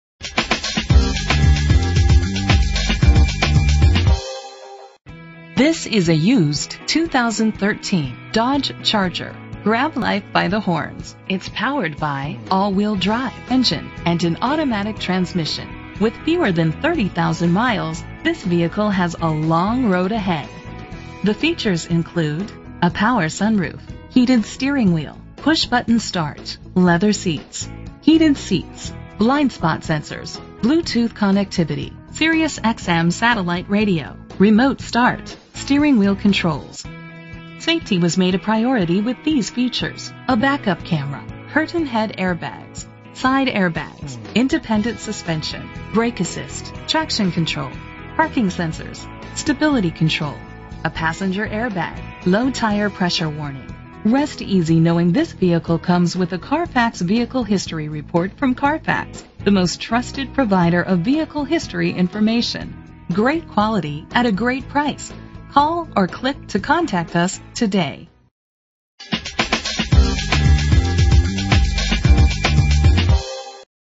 2013 Dodge Charger - Syracuse NY
Year: 2013
Make: Dodge
Model: Charger
Trim: R/T
Engine: 5.7 L, 8 cylinder
Transmission:
Color: White
Mileage: 28173
Address:
1230 West Genesee Street